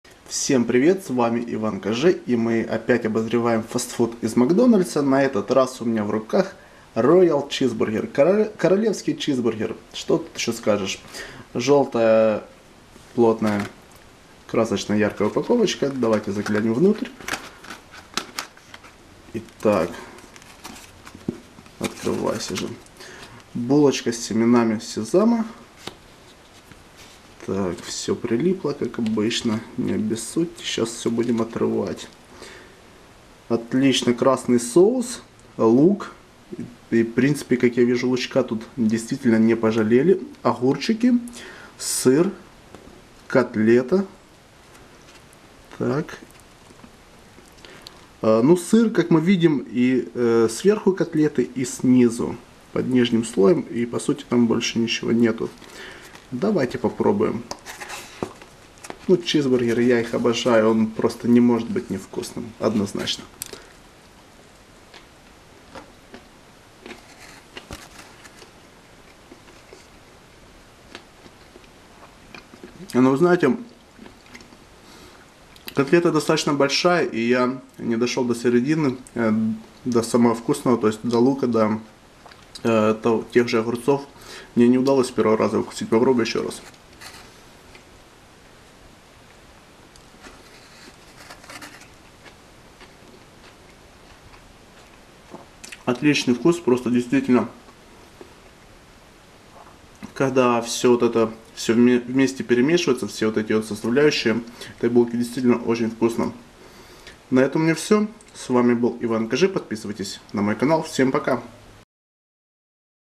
Роял Чизбургер Макдональдс Обзор McDonalds Royal Cheeseburger
Обзор фастфуда из сети МакДоналдс от Иван Кажэ, в этом видео смотрите обзор Роял Чизбургер МакДоналдс. Больше вкусных обзоров вы найдете на моем канале Иван Кажэ.